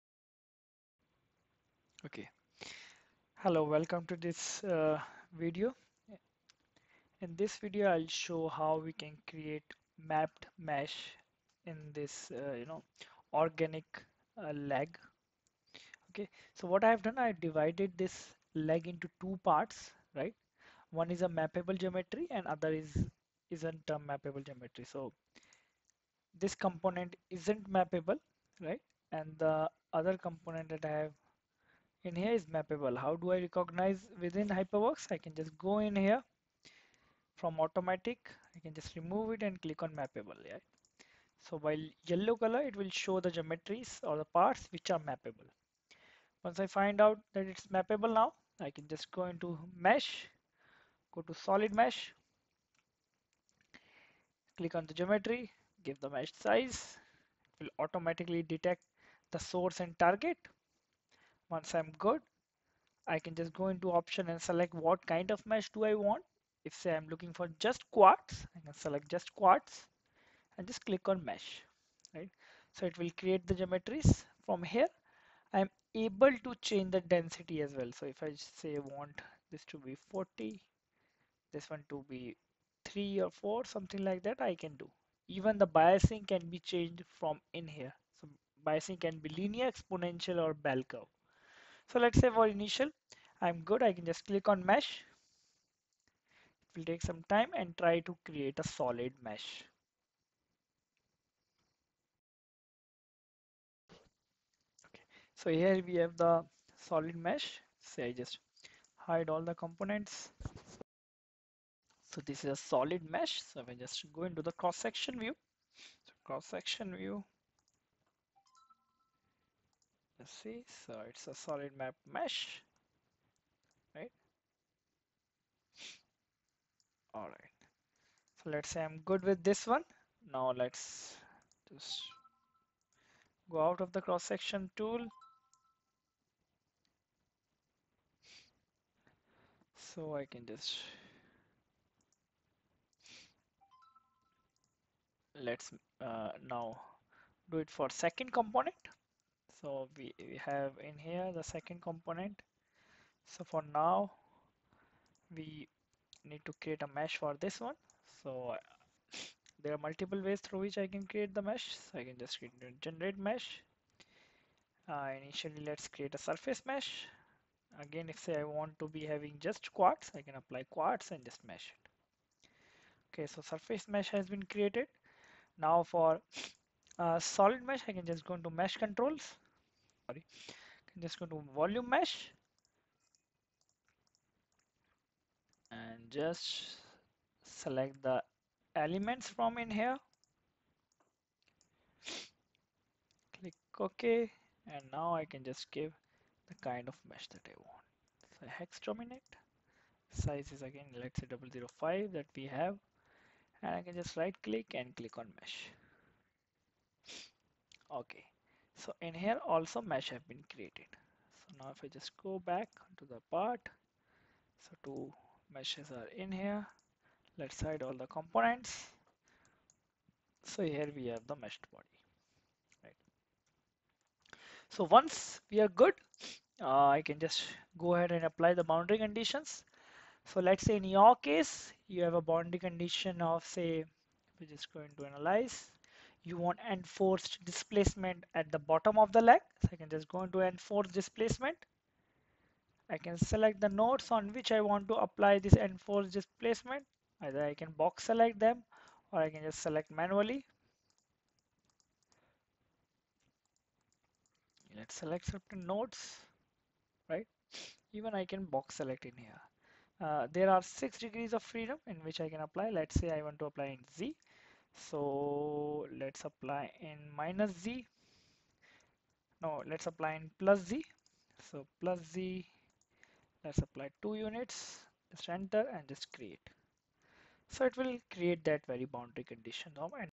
Mapped Mesh using HyperMesh
Watch HyperMesh in action as it effortlessly creates a perfectly mapped mesh for an organic leg - achieving unparalleled accuracy and efficiency in mesh generation!

Please feel free to post questions in the comment section.